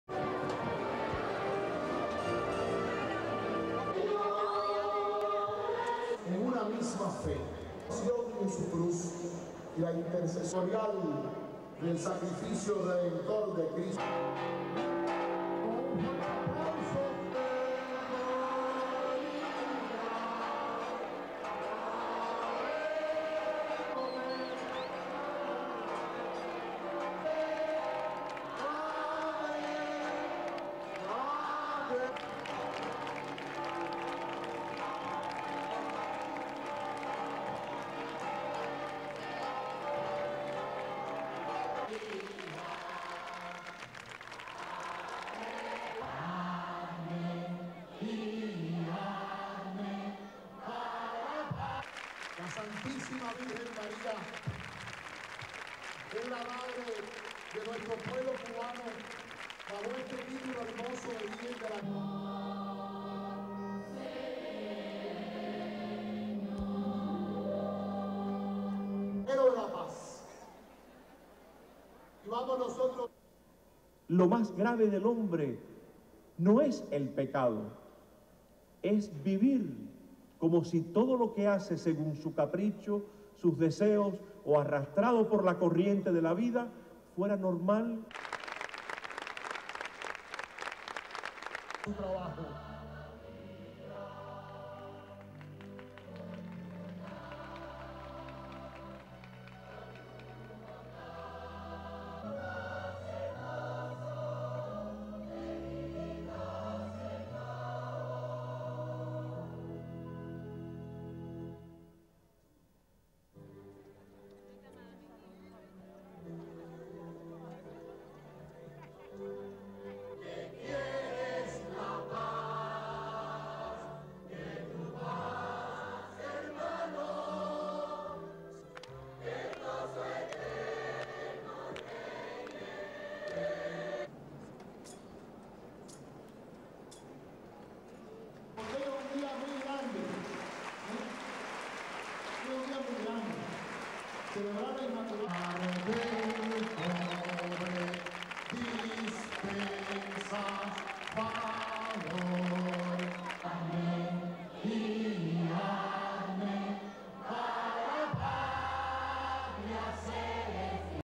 CUBA: SERIES OF OPEN AIR MASSES LAY GROUNDWORK FOR POPE VISIT
(9 Dec 1997) Spanish/Nat

A historic series of open-air Masses in Cuba has helped lay the groundwork for Pope John Paul II's visit next month.

Cardinal Jaime Ortega ended the string of Masses late Monday by insisting on the close links between Cuban patriotism and religious faith - a relationship not always obvious to Cuba's communist officials, who abandoned official atheism only six years ago.

Speaking in front of a large crucifix and the flags of Cuba and the Vatican, Cardinal Jaime Ortega held a Mass Monday on the steps of Havana's cathedral.

After the communist revolution in 1959, public displays of religious faith and worship were forbidden in Cuba.

But following the improvement of relations between Pope John Paul II and Cuban President Fidel Castro, the government has made concessions which have allowed greater religious freedom.

Ortega recalled that Cuba's 19th century independence heroes, the Mambis, had venerated the Virgin of Charity of Cobre, whom the Pope will honour as Cuba's patron saint.

Some two-thousand worshippers faced Ortega - many cheered and waved white handkerchiefs as an image of the Virgin was paraded through the crowd.

It was one of the larger crowds in the series of 11 Masses - seven of them held in public, outdoor areas for the first time in decades of communist rule.

And the Cardinal preached a simple message.

SOUNDBITE: (Spanish)
"The worst thing about people is not sin. It's as if it was normal to live according to your whims, your desires or being dragged along by the current of life."
SUPER CAPTION: Cardinal Jaime Ortega

Permission for the Masses showed government willingness to cooperate in making the late January visit of the Pope a success -
though suspicions obviously still linger on the part of some officials.

At least one Mass planned for the streets was forced indoors when officials at the last minute denied permission.

Some early Masses suffered power outages common to Cuba. But the
last few went smoothly, with no interference from police.

The government sees the papal visit in part as another break in U-S attempts to isolate Cuba and has stressed the Vatican's opposition to the U-S trade embargo of the island.

Church officials say they hope the visit will help strengthen religious faith and moral values, and ease the restrictions still facing churches on the island.

They have tried to dampen sometimes exuberant hopes that the visit will help the island's economic hardship.

Government officials have promised to use public transport to bring worshippers to papal Masses and to make space available in the official news media - the only kind here - to publicise the visit.